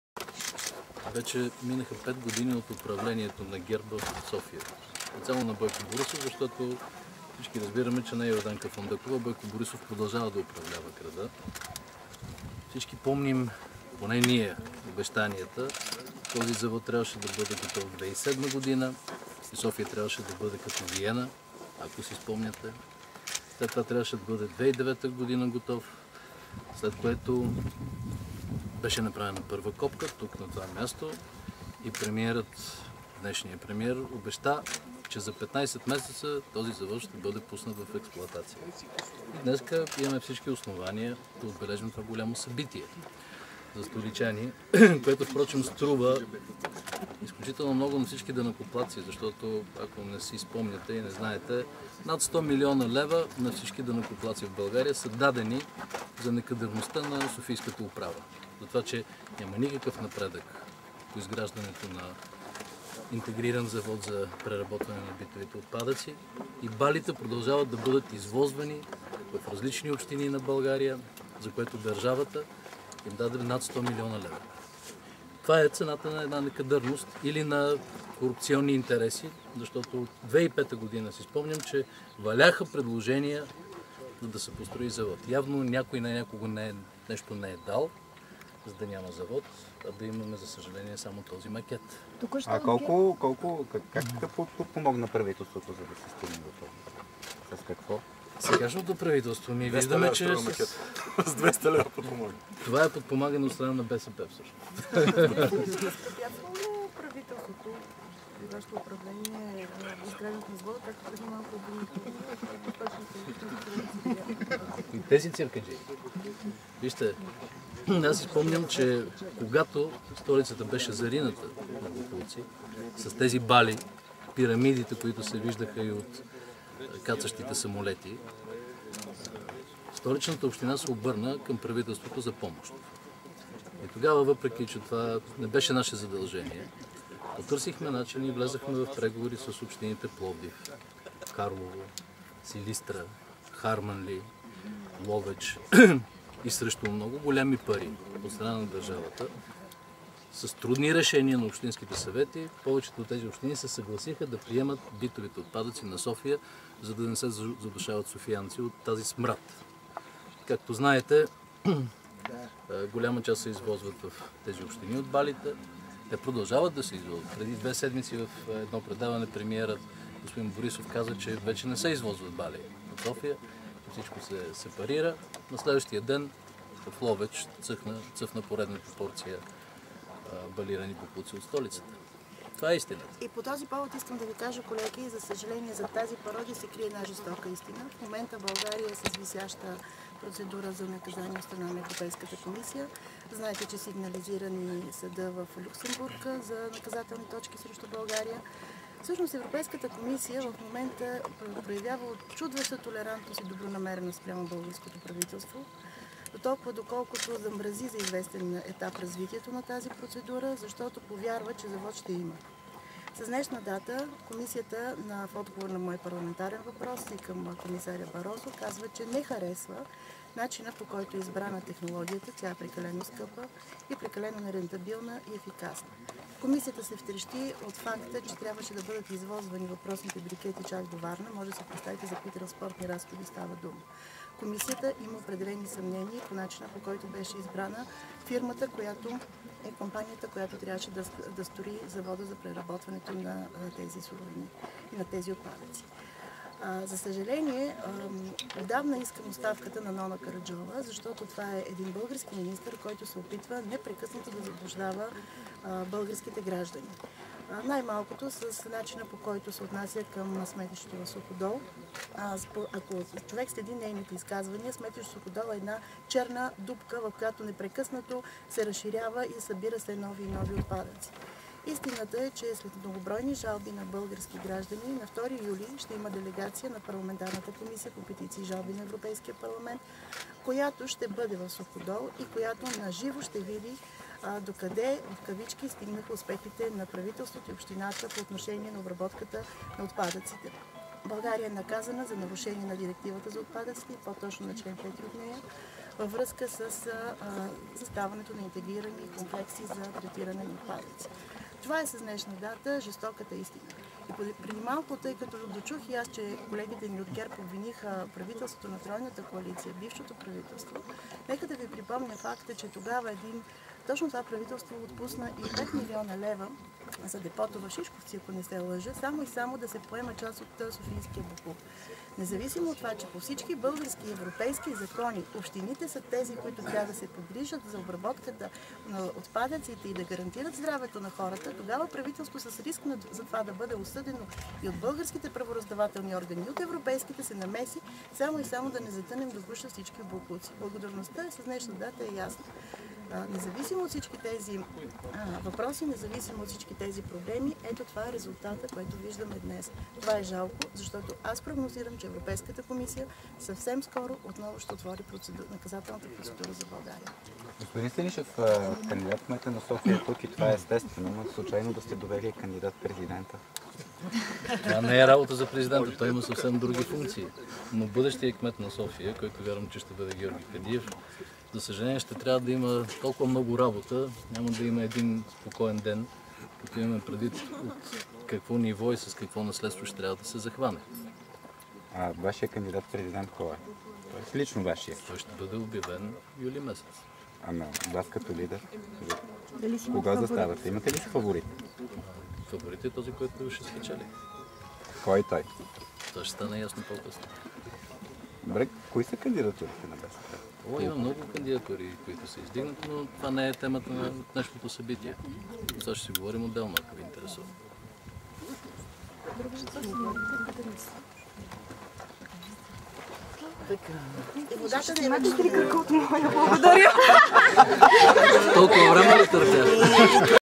Завод за отпадъци - откриване - Станишев - Йотова
Не Йорданка Фандъкова, а Бойко Борисов реално продължава да управлява София. Това каза председателят на НС на БСП Сергей Станишев, който присъстваше на официалната церемония по "откриването" на непостроения от Бойко Борисов завод за битови отпадъци, в местността "Садината" край с. Яна.

По думите на Станишев, над 100 млн. лв. са дадени за некадърността на софийската управа и за това, че няма никакъв напредък по изграждането на интегриран завод за преработване на битови отпадъци. Станишев напомни, че балите продължават да се извозват в различни български общини.

„От 2005г. валяха предложения да се строи завод, но явно някой на някого нещо не е дал, за да няма завод", -- коментира лидерът на БСП. Той напомни, че когато София беше зарината с боклук, Столичната община се обърна към правителството за помощ. Въпреки, че не беше задължение на държавата, се проведоха преговори с различни общини и срещу сериозни разходи от страна на държавата тежката ситуация беше преодоляна.

Евродепутатът Илияна Йотова изрази опасенията си, че скоро ЕК отново ще отвори наказателната процедура за България. Тя напомни, че в момента страната ни е с висяща процедура за наказание от страна на ЕК, като Комисията проявява учудваща толерантност спрямо българското правителство, замразявайки за известен етап тази процедура.

„Причината е, че повярва, че ще има завод", обясни Йотова. По думите й Комисията не харесва начина, по който е избрана технологията и има съмнения и за начина, по който е избрана фирмата, която да строи завода. „Отдавна искам оставката на Нона Караджова, защото това е министър, който непрекъснато се опитва да заблуждава българските граждани, най- малкото по отношение на Суходол", заяви Йотова. Тя обяви, че на 2 юли на място в Суходол ще дойде делегация на парламентарната Комисия по петиции и жалби на ЕП.